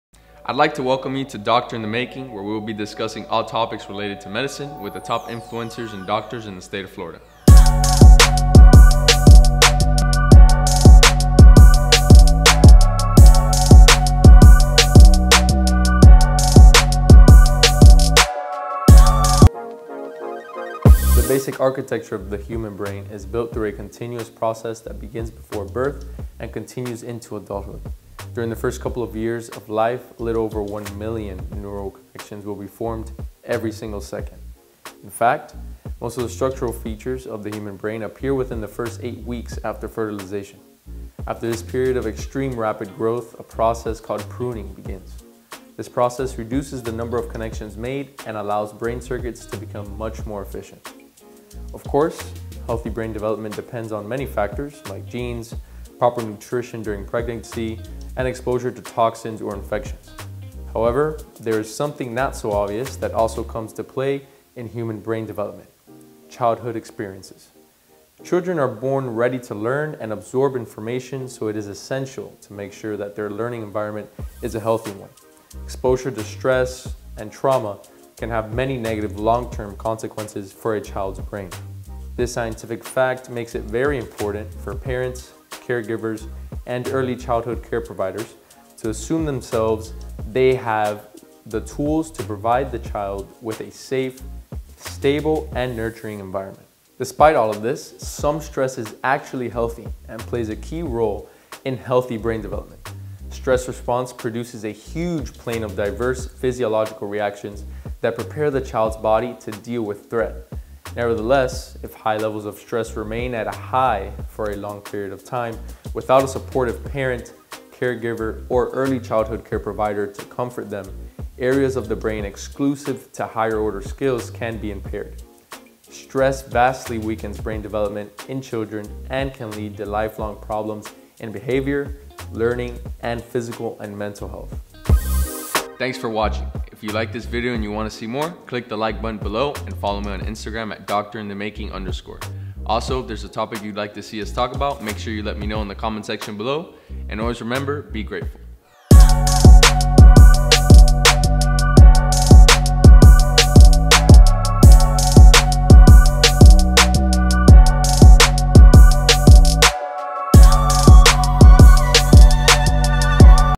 Brain Development with Emanuel Arias
In this video, I will be discussing the development and architecture of the human brain.